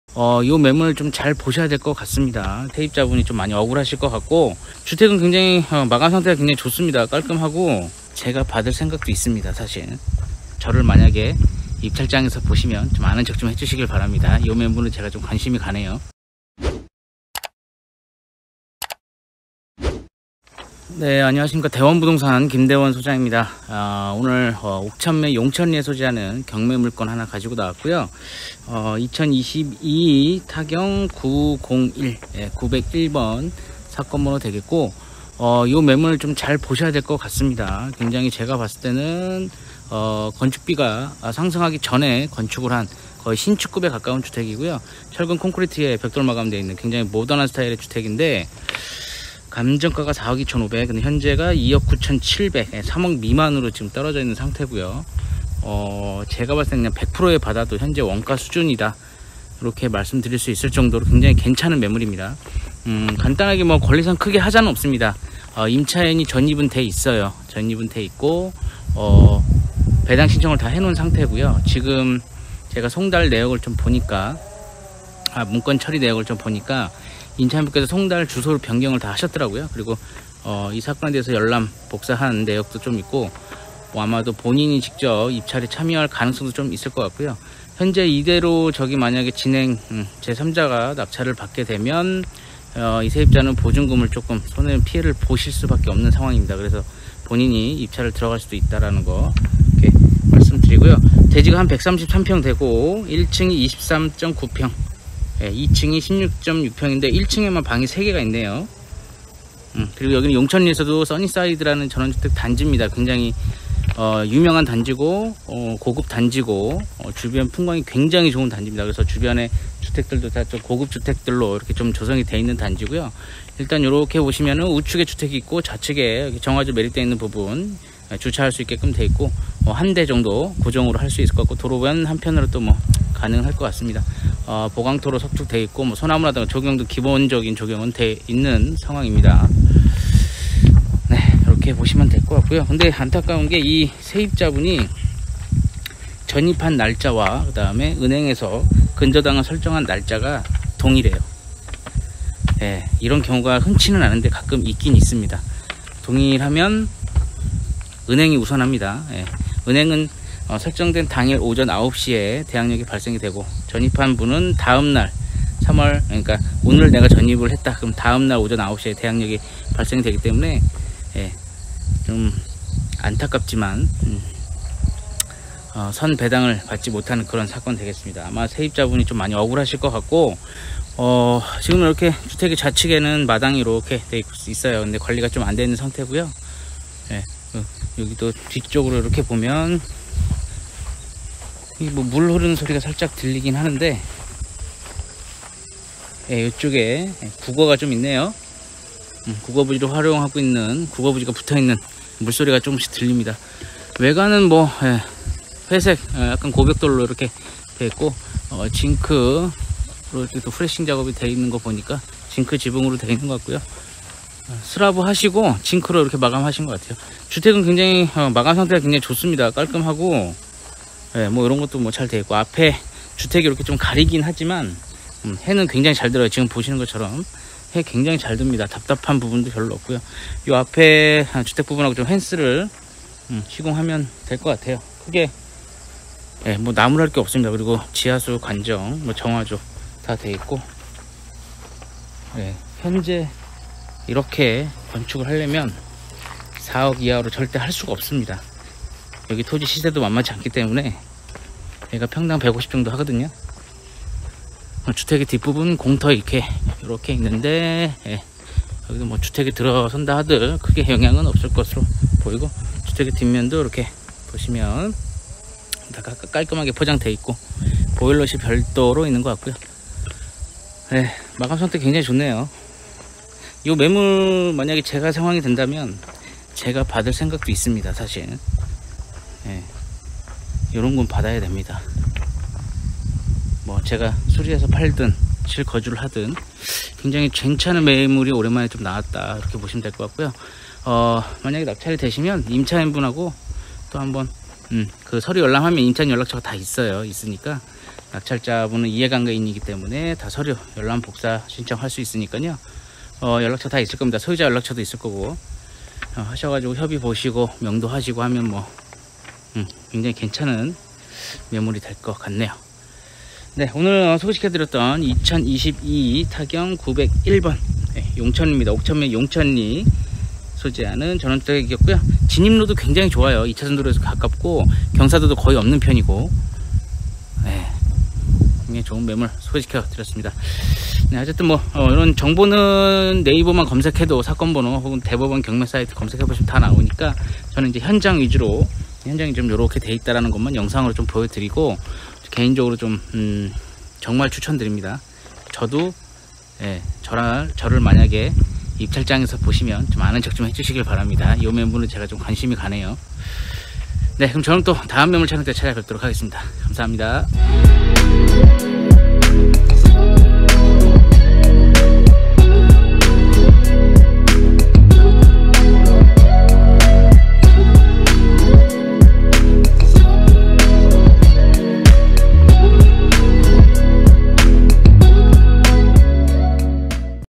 양평전원주택경매 / 최저가2억9천대 / 2022타경901 / 옥천면 용천리 2억대 경매매물 / 매각예정일 10월19일 / 양평부동산경매 / 대원부동산
경기 양평군 옥천면 용천리 176-192(단독주택)
여주6계 2022-901 / 임의
용도 단독주택 감정기관 대일감정(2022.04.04) 채권자 양서농협
매각방법 일반  감정가 425,208,000원 채무자 이정숙
접수일자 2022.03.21 최저가 297,646,000원 소유자 이정숙
유찰/진행 1회/1회 최종결과 예정 ( 물건매각일: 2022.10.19 )

소재지 / 특성면적 감정가 외 기타 정보
총감정금액: \425,208,000
  · 토지감정: \238,680,000
  · 건물감정: \186,528,000
표준공시지가 : \300,500
개별공시지가 : \288,400
감정지가 : \540,000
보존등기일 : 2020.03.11

보전관리지역,배출시설설치제한지역,공장설립승인지역,수질보전특별대책지역
가축사육제한구역
자연보전권역

* 철콘조철콘지붕
* 용천3리마을회관북동측원거리위치
* 부근단독주택,임야등혼재지역
* 차량접근가능,제반대중교통사정보통
* 인근버스(정)소재
* 보일러설비
* 남동측하향의사다리형완경사지
* 북동측6m도로접함

대지
· 442.00m²(133.71평)
건물내역
· 1층 주택 79m²(전용:23.90평): 방3, 욕실3
· 2층 주택 55m²(전용:16.64평)
전체 총 2층

사무소명칭/대표 : 대원공인중개사무소 / 대표 김대원
중개사무소 등록번호 : 제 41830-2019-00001 호
사무소위치 : 경기 양평군 양평읍 중앙로 107 3층 301호